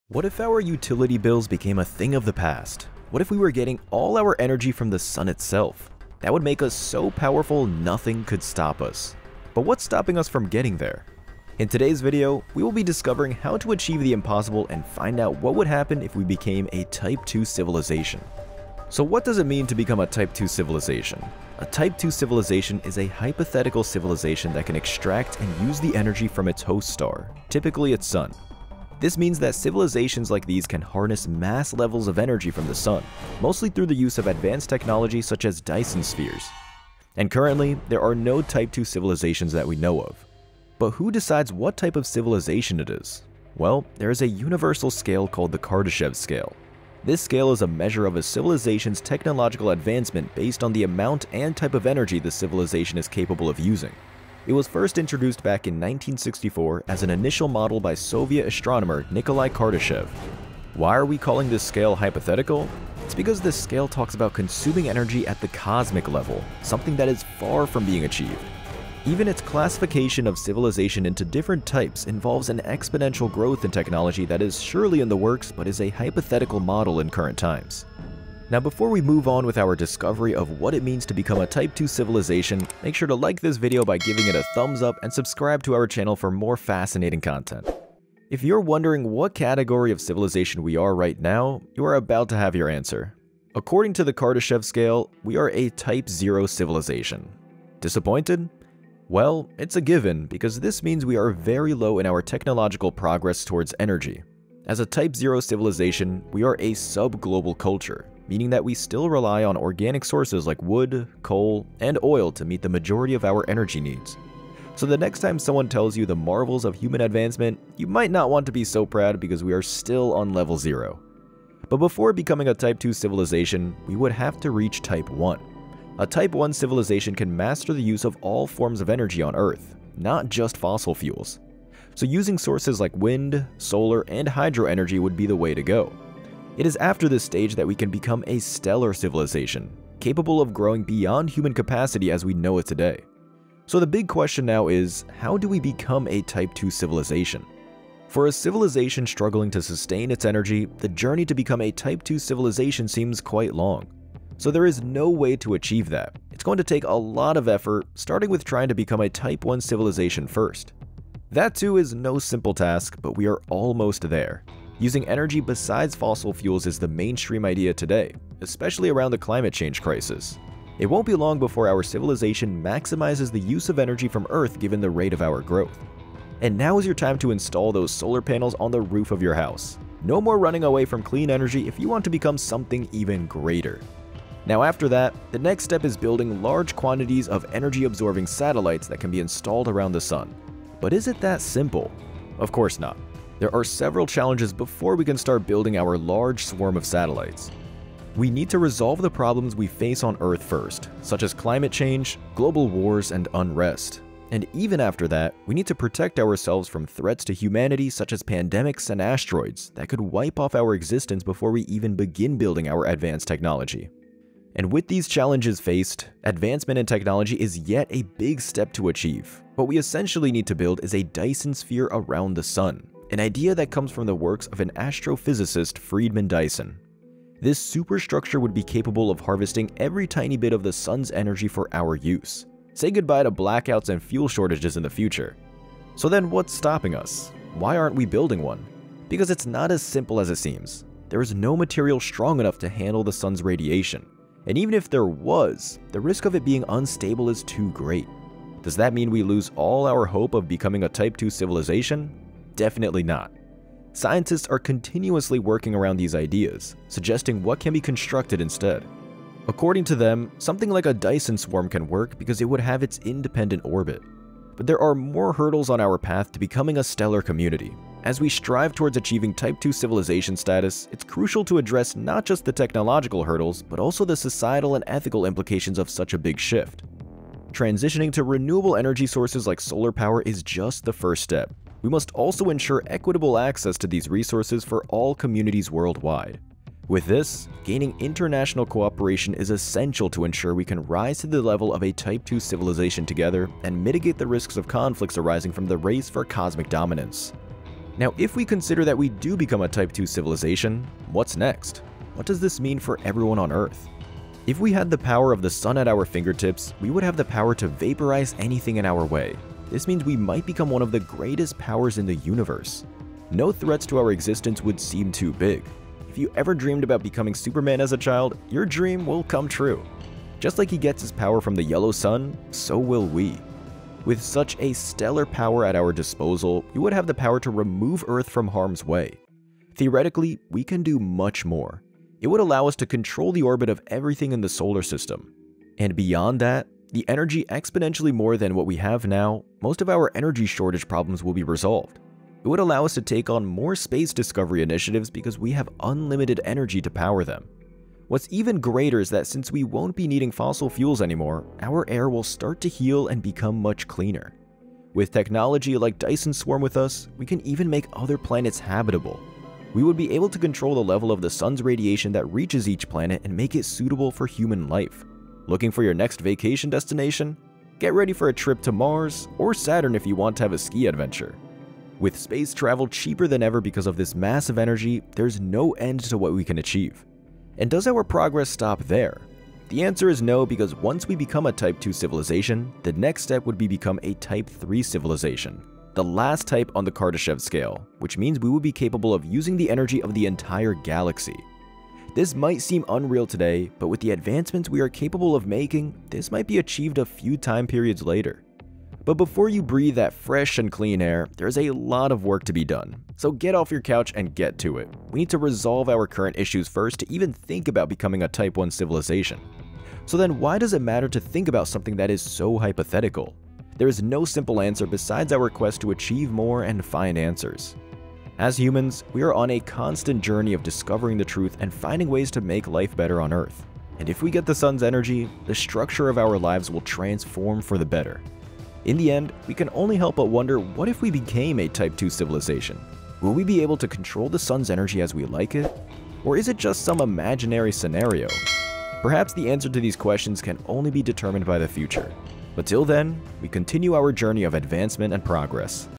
What If We Became A Type II Civilization?
Unlock the secrets of a Type II civilization in this captivating video exploration! Delve into the realms of the Kardashev scale and envision a future where humanity harnesses the boundless energy of our sun. From the humble beginnings of a Type 0 civilization to the lofty aspirations of Type II mastery, discover the transformative journey that lies ahead. Join us as we unravel the technological marvels and daunting challenges on the path to becoming a stellar civilization. From Dyson spheres to Dyson swarms, explore the innovative solutions that could propel us into a new era of cosmic power. Imagine a world where utility bills are a relic of the past, and humanity stands poised to shape the very fabric of our solar system. Embark on this visionary quest and dare to dream of what awaits us beyond the horizon of possibility.